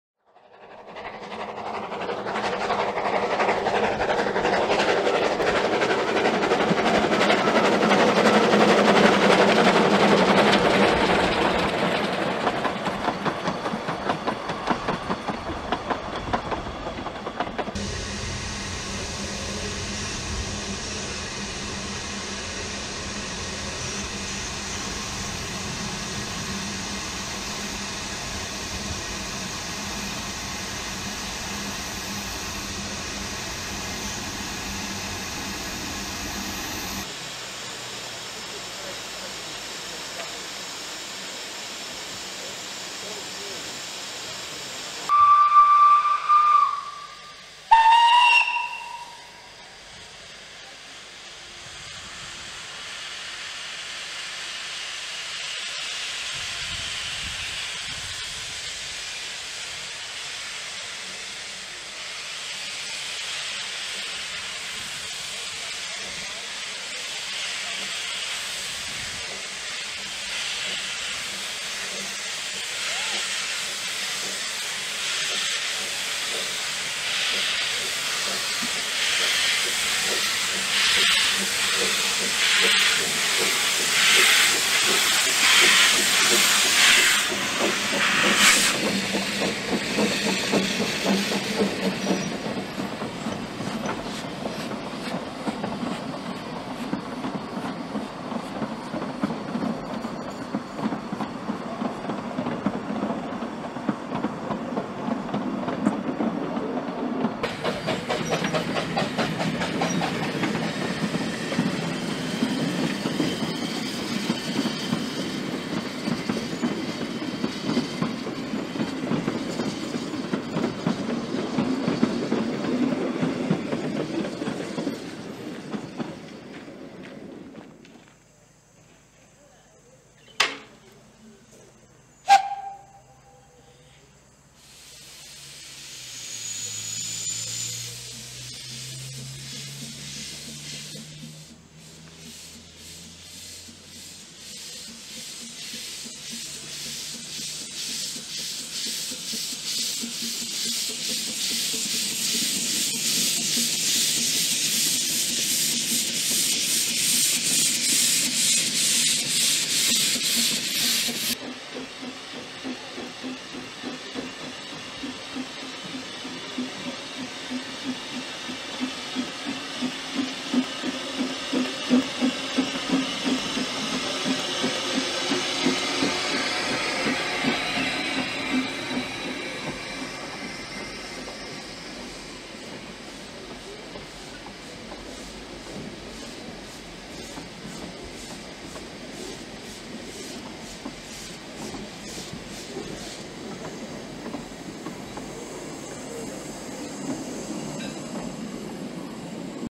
Blast from the Past - Some highlights from 2018 Autumn Gala
Trevor Garnham over at @TAGVideoProduction has done it again - he's provided us with some sights and lovely sounds of a few years ago, when Lyn and Lyd ran together and the sun shone bright over Woody Bay.

Do you have some footage you'd like to share with us?